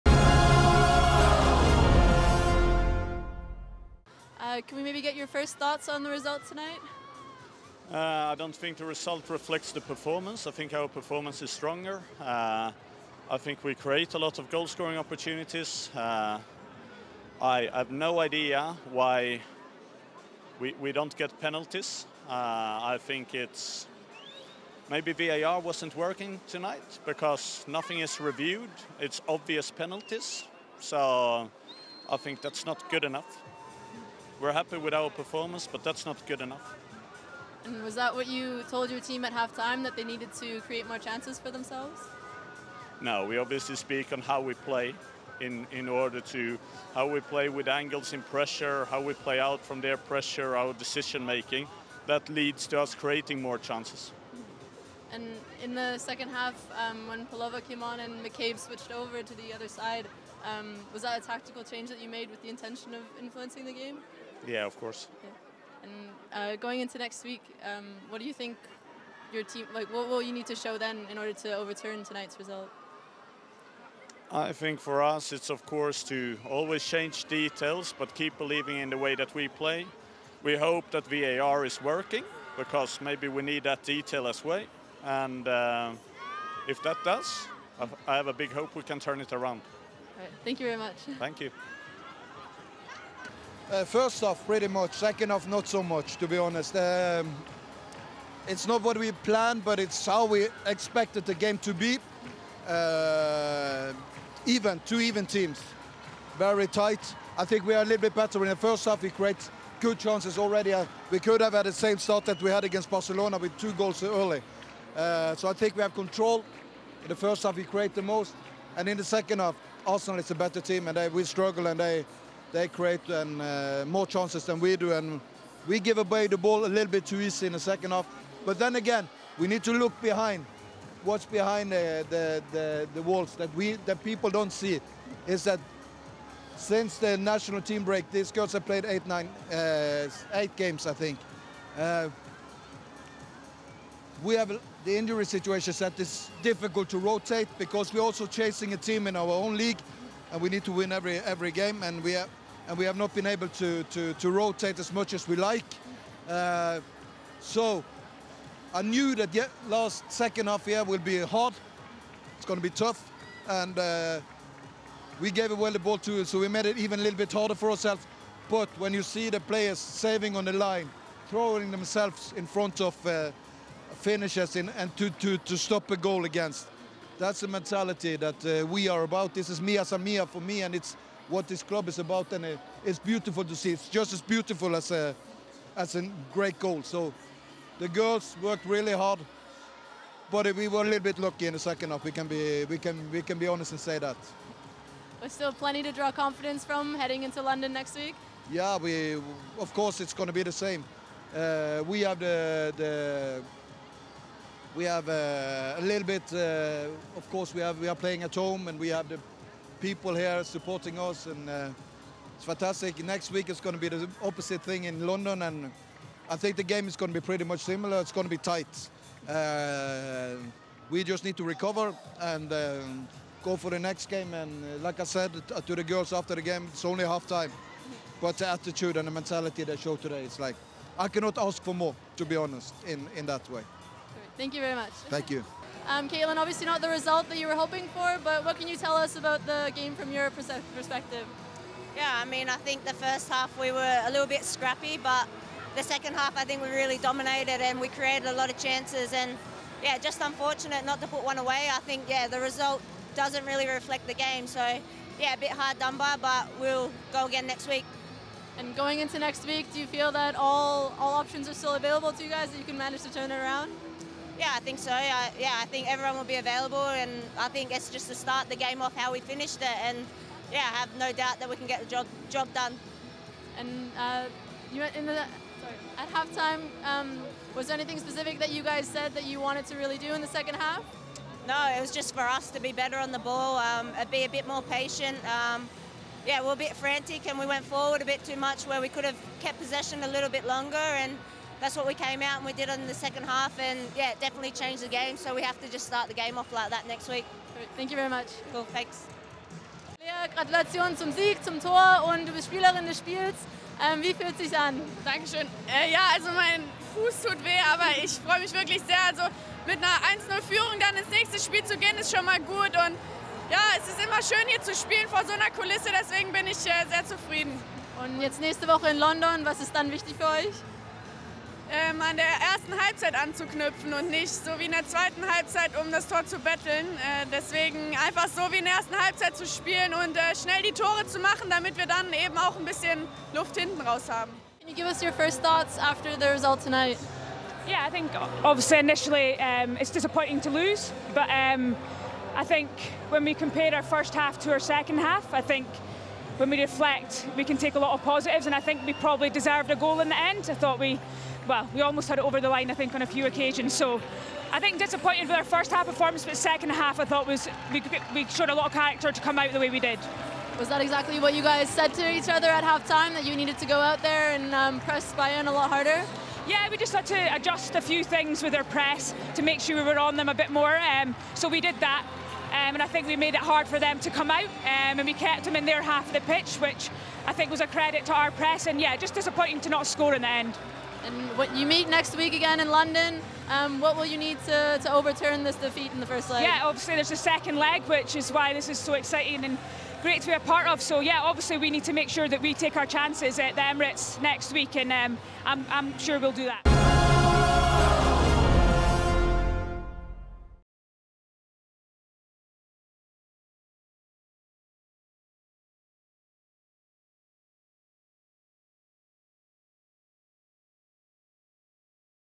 Eidevall Struggles To Explain Arsenal's Blank Against Bayern, Straus Salutes Mia San Mia Spirit
Jonas Eidevall was left highly frustrated by Arsenal's defeat against Bayern Munich but his counterpart Alexander Straus was delighted with his troops' efforts after a gruelling fixture schedule.